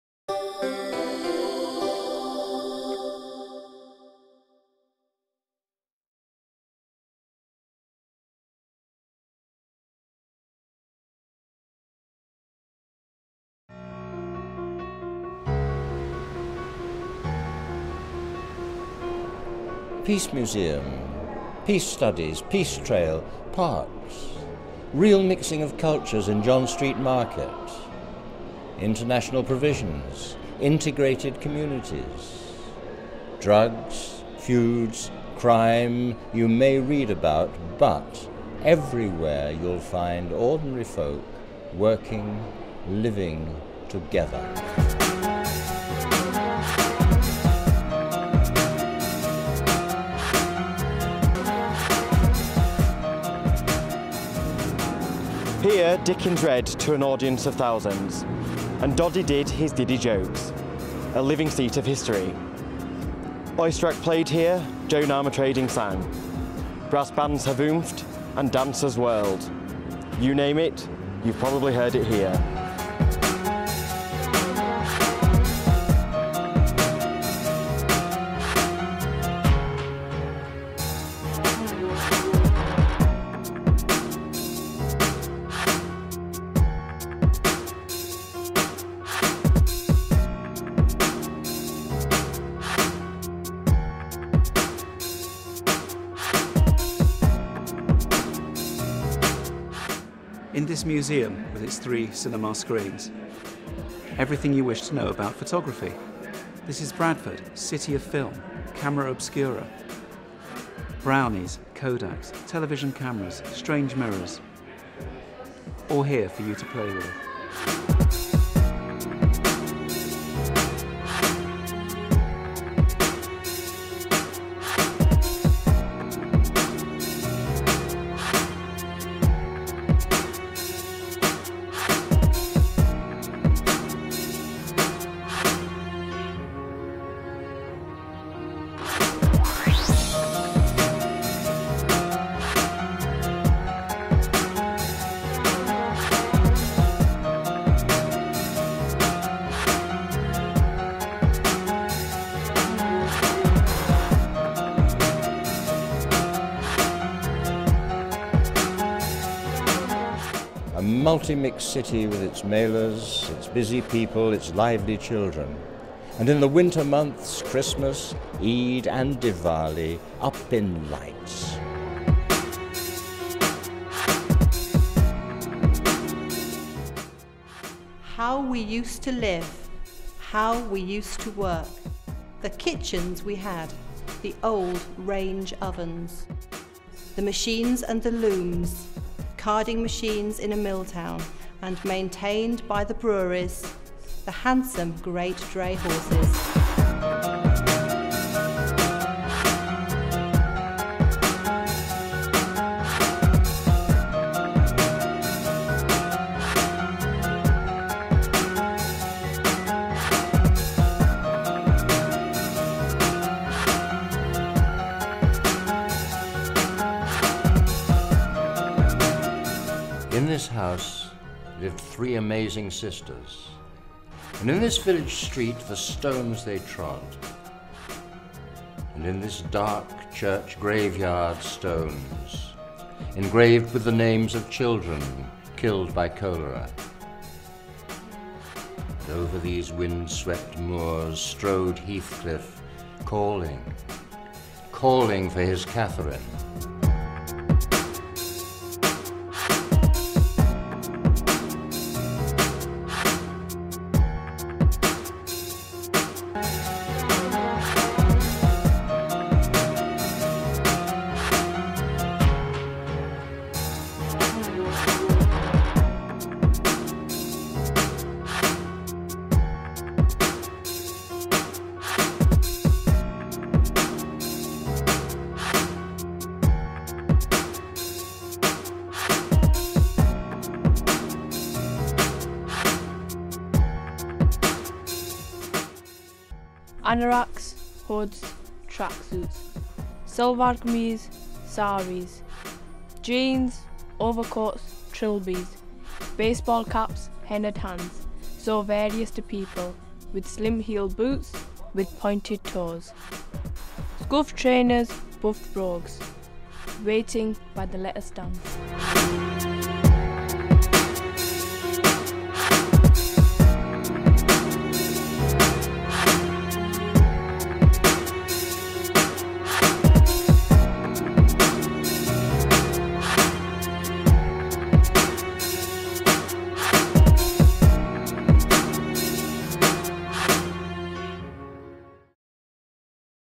Bradford Poem
Students from the University of Bradford have been busy producing short films for Bradford's Big Screen.

The films highlight some of the cultural gems within the Bradford District including the Industrial Museum,  Bradford 1 Gallery, Impressions Gallery and St Georges Hall as well as promoting some of the stunning landscape in the area including a stretch of the Leeds / Liverpool canal and Ilkley.

Bradford Council, the BBC, the National Media Museum and the University of Bradford have all worked together to brief and mentor the 40 students.

The students, from Bradford Media School, will be assessed on their work as part of a module that will count towards their degree.